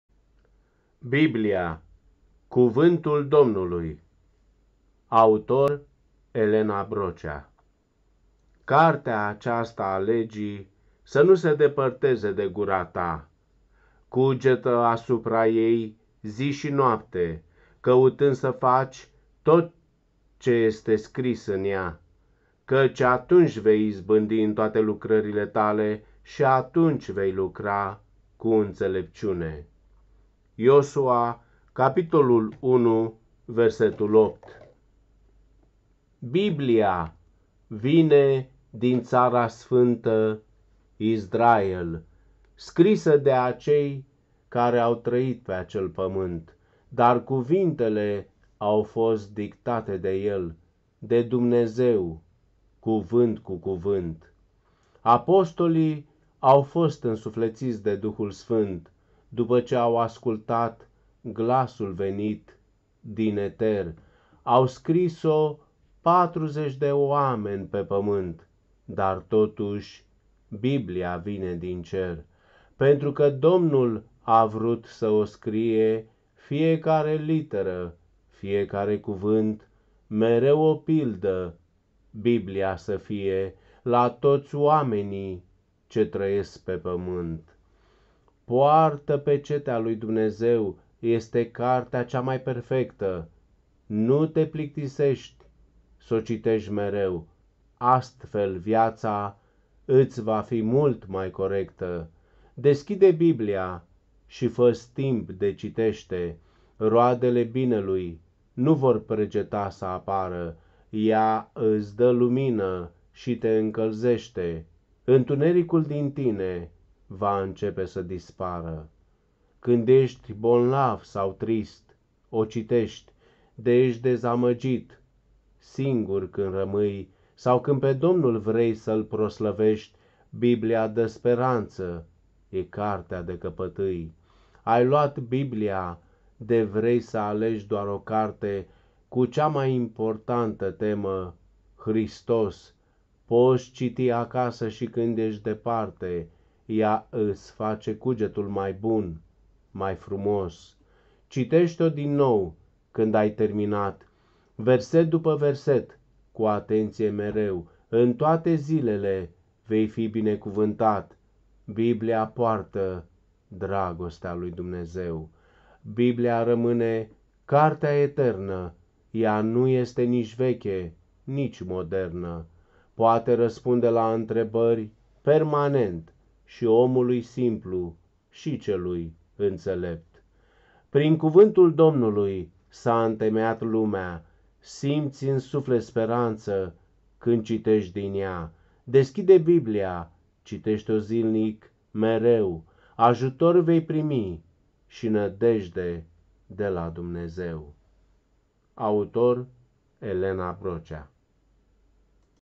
Biblia - Cuvantul Domnului - Autor : Elena Brocea - "Vorbeste, Doamne, caci robul Tau asculta."
BIBLIA- CUVÂNTUL DOMNULUI.
     Cartea asta a Legii să nu se depărteaze de gura ta, cugetă asupra ei, zi și noapte, căutând să faci ce este scris în ea, căci atunci vei izbândi în toate lucrurile tale și atunci vei lucra cu înțelepciune.
              ( Iosua 1:8 )

Biblia, vine din Țara Sfânta, Israel,
Scrisă de acei care au trăit pe acel pământ.
Dar cuvintele au fost dictate de El,
De Dumnezeu, cuvânt cu cuvânt.

Apostolii au fost însuflețiți de Duhul Sfânt,
După ce au ascultat glasul venit din eter.
Au scris-o 40 de oameni pe pământ,
Dar totuși, Biblia vine din cer.

Pentru că Domnul a vrut sa o scrie,
Fiecare literă, fiecare cuvânt.
Mereu o pildă BIBLIA să fie,
La toți oamenii ce trăiesc pe Pământ.

Poartă pecetea lui Dumnezeu;
Este cartea cea mai perfectă ;
Nu te plictisești, s-o citești mereu !
Astfel viața, îți va fi mult mai corectă.

Deschide Biblia și fă-ți timp de citește,
Roadele binelui, nu vor pregeta să apară.
Ea îți dă lumină și te încălzește,
Întunericul din tine, va începe să dispară.

Când ești bolnav sau trist, o citești,
De ești dezamăgit, singur când rămâi
Sau când pe Domnul vrei să-L proslăvești,
Biblia dă speranță, e cartea de căpătâi.

Ai lua Biblia, de vrei sa alegi doar o carte,
Cu cea mai importantă temă - Hristos!
Poți citi acasă și când ești departe,
Ea îți face cugetul mai bun, mai frumos.

Citește-o din nou, când ai terminat,
Verset după verset, cu atenție mereu
In toate zilele, vei fi binecuvantat,
Biblia poartă dragostea lui Dumnezeu.

Biblia rămâne cartea eternă,
Ea nu este nici veche, nici modernă.
Poate răspunde la întrebări permanent,
Și omului simplu și celui înțelept.

Prin Cuvântul Domnului, s-a întemeiat lumea,
Simți în suflet speranță, când citești din ea.
Deschide Biblia, citește-o zilnic, mereu !
Ajutor vei primi și nădejde de la Dumnezeu.
         Autor Elena Brocea